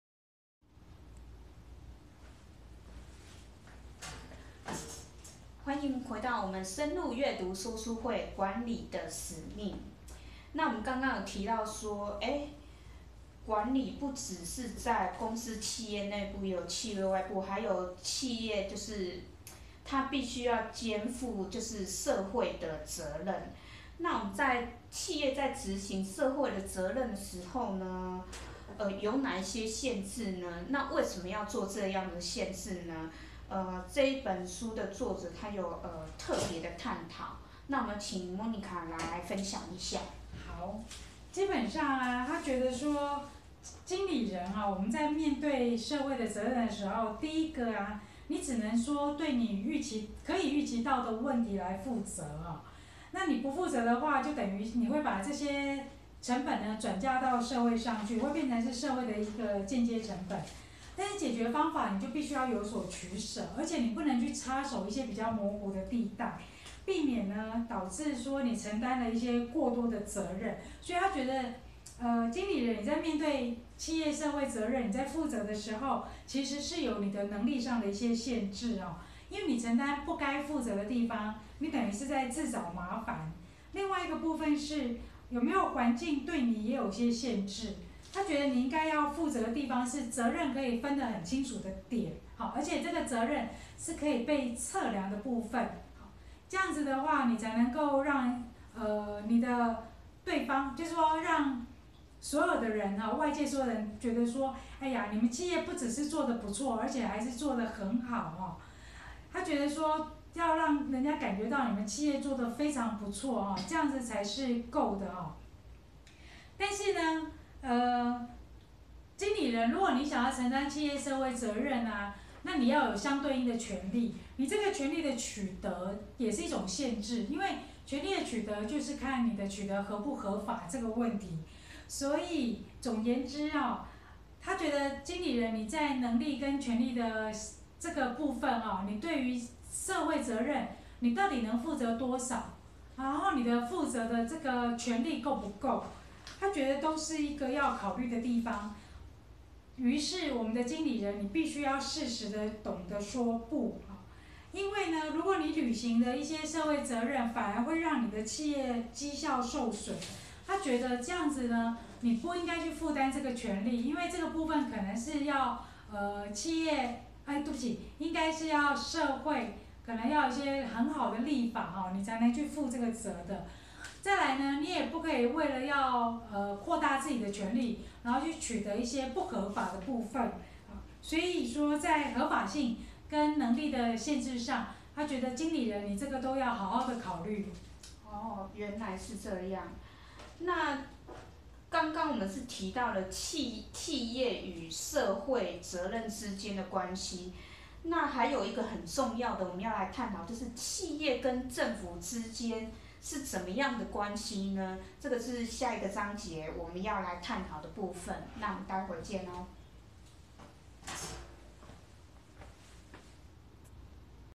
管理的使命－２０ 社會責任的限制
原文即厚達八百多頁《管理的使命、實務與責任》！是管理學大師彼得.杜拉克三十七本著作中，最重要的一本，是管理學的聖經。

內文的經典之處實在太多了，很難用短時間去說完整，只能拋出部分磚頭來，引誘大家自行去閱讀此書。

（本影片目的在為拋磚引玉，更多詳細內容請自行閱讀該書籍）

書名：杜拉克：管理的使命
出版：2002/02/05　天下雜誌出版社
說書人：深入閱讀學會